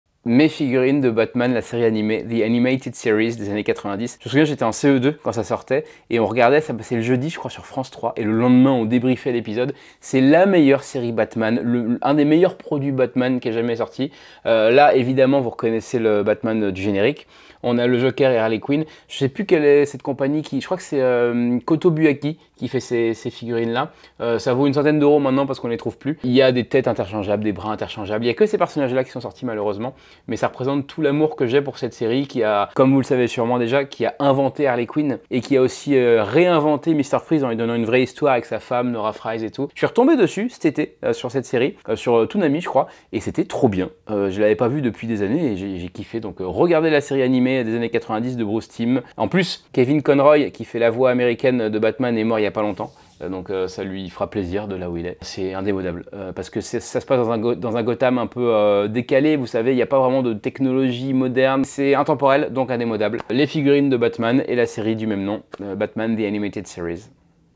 Mes figurines Batman The Animated Series !
@FreakyHoody présente ses trop rares figurines Batman the animated series par kotobukiya !

réalisation : @therezim_ Joana Zozime